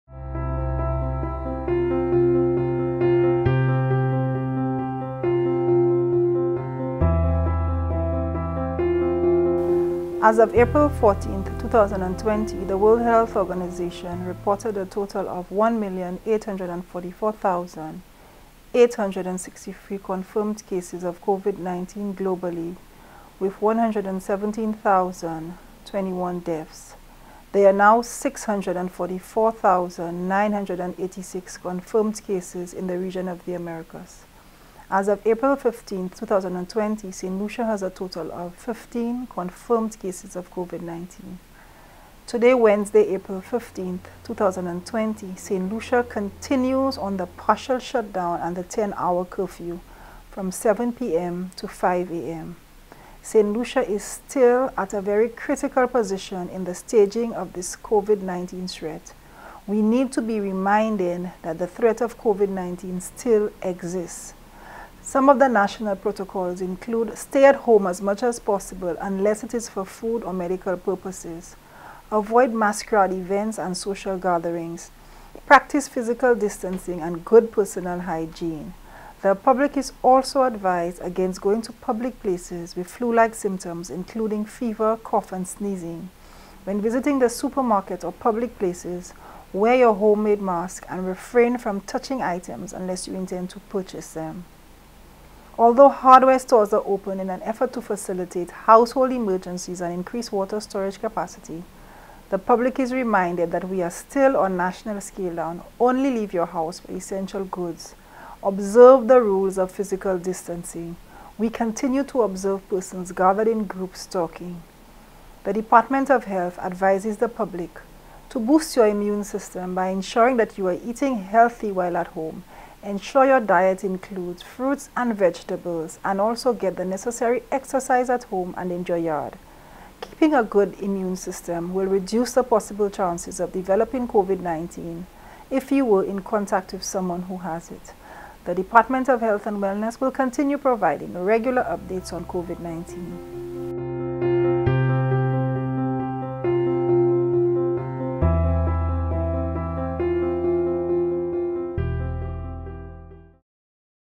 Saint Lucia still on partial shutdown
The public is reminded that Saint Lucia is still on national scale down. Only leave your house for essential goods.